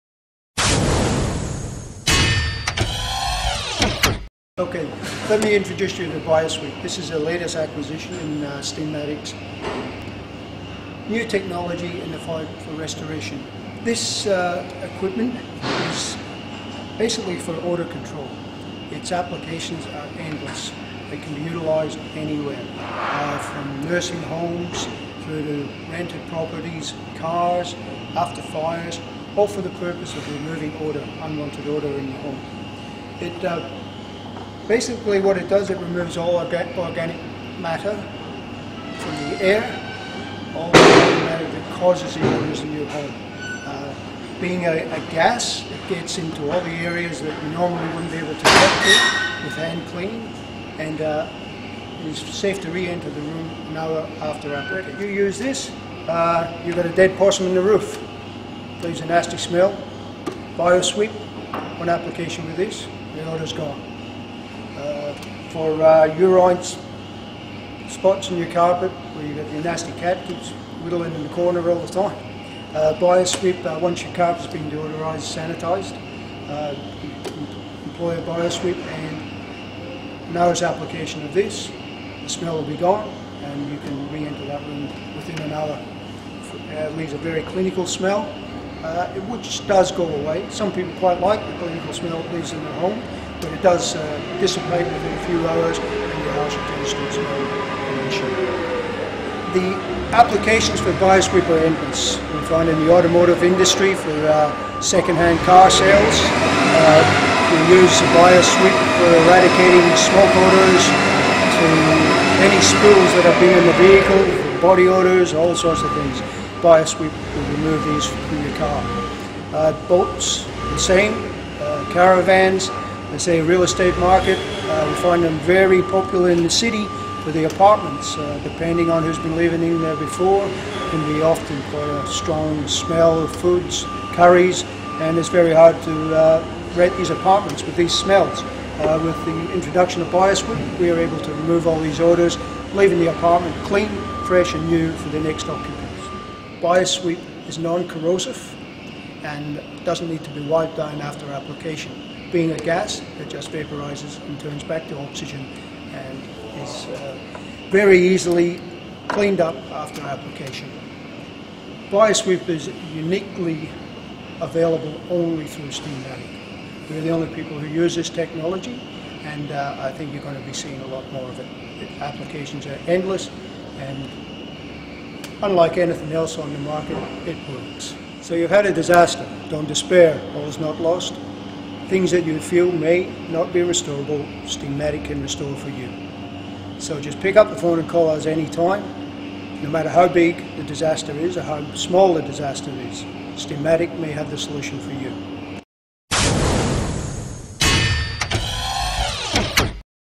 Bio-Sweep-Odour-Removal-Steamatic.qt
Our equipment's unique, five-stage process, called PHOCATOX, combines the latest in airborne and surface contamination destruction technology. Unlike any other equipment available on the market today, the BioSweep creates a hydroxyl radical plasma gas inside of the unit's intense sterilizing chamber, producing and exporting very high levels of vaporized hydrogen peroxide and purified ozone out into treatment areas for effective surface decontamination.